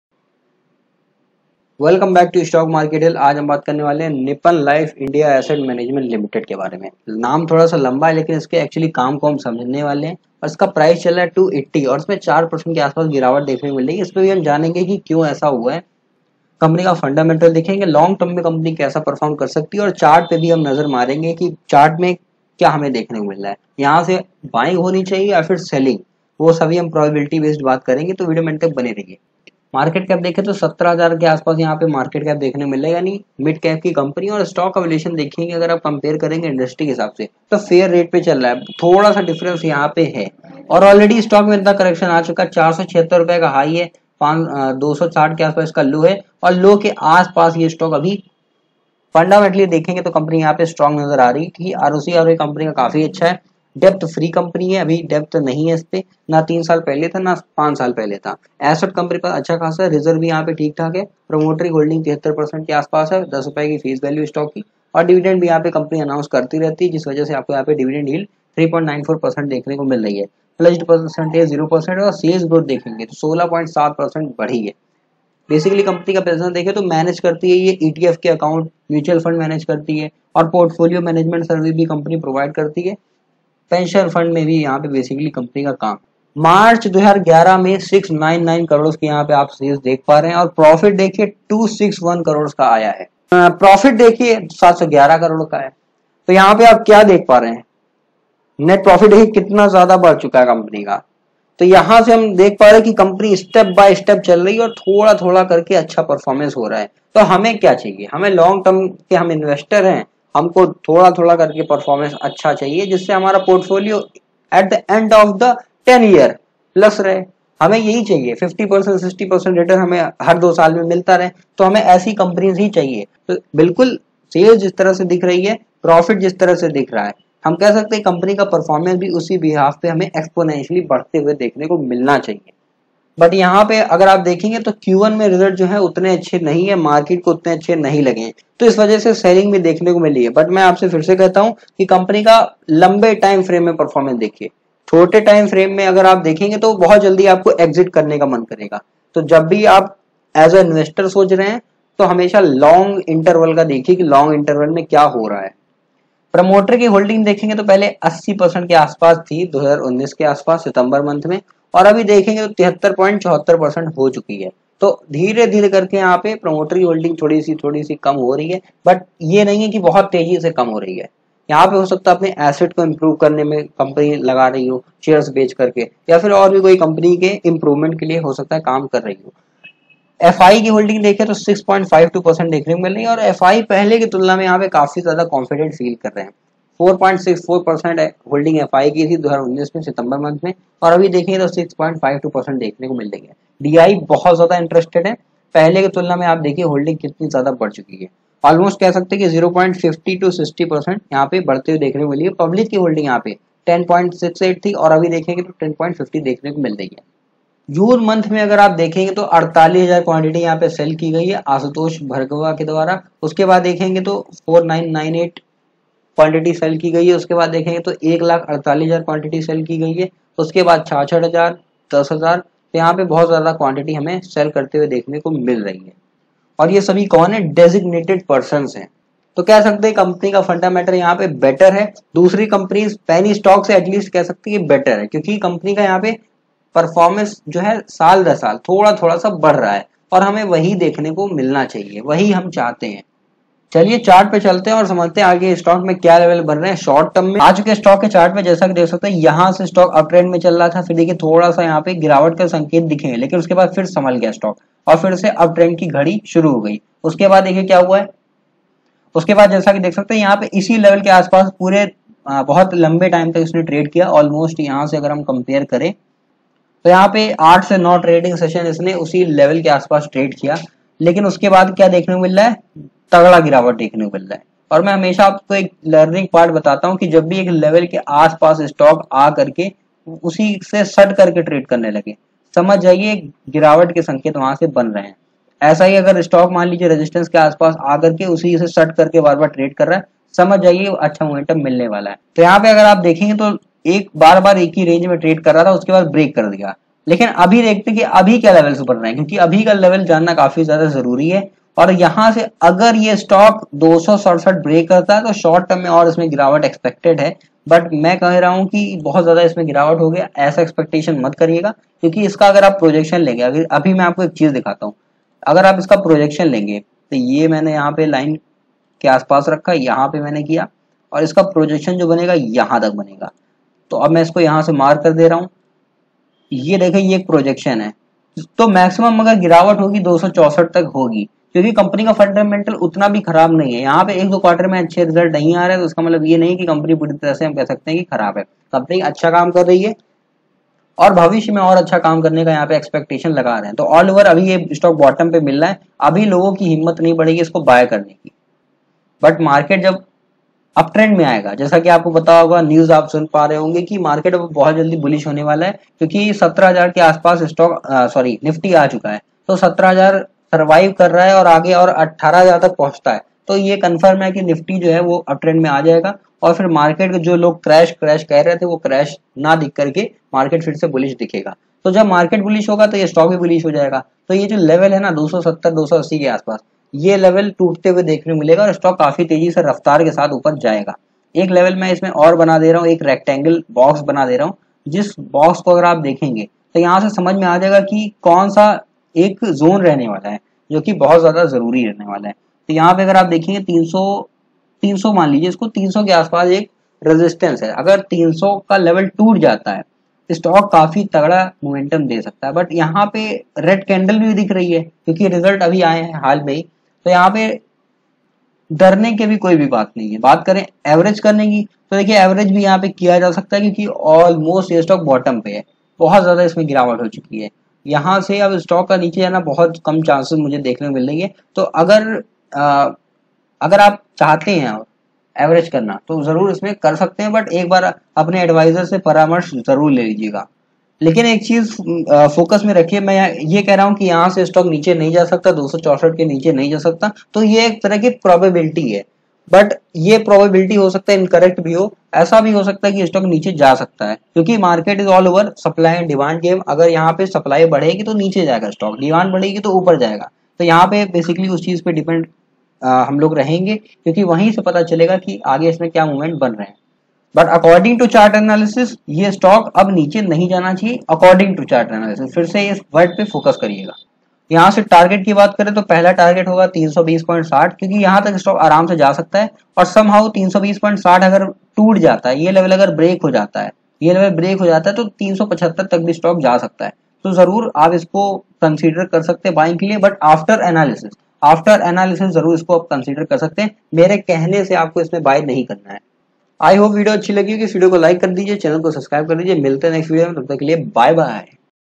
Nippon life asset management share latest news || Stock Market Hill
In this video I Have done fundamental analysis as well as technical analysis on Nippon life asset management.